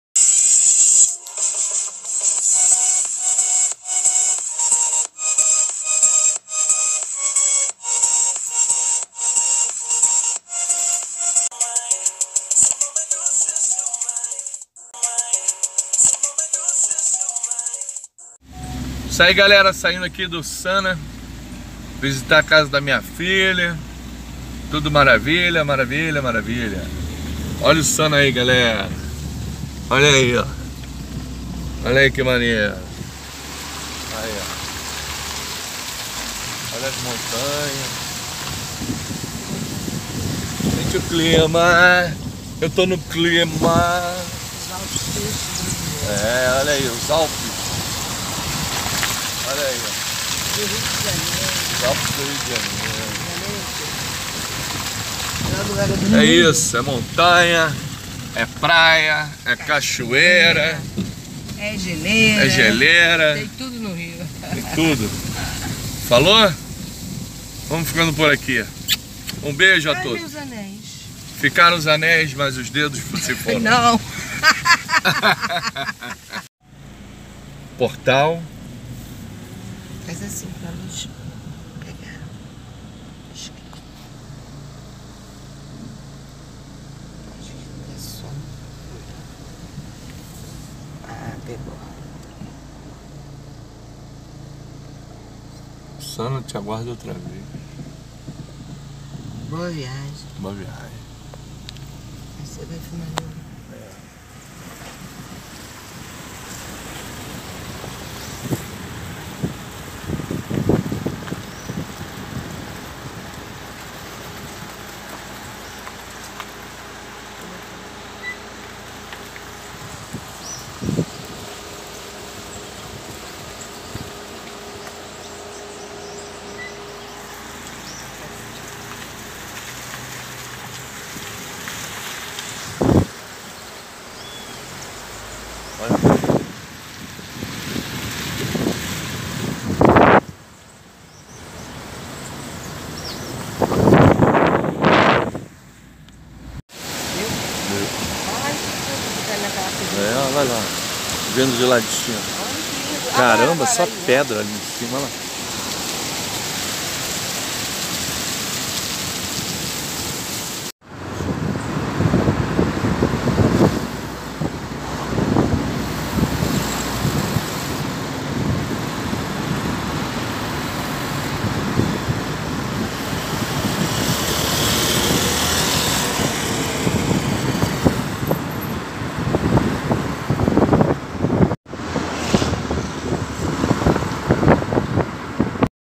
SANA A SAQUAREMA TRIP
sao joao no sana a turma da perna de pau fez a festa @sabrinadianovsky2384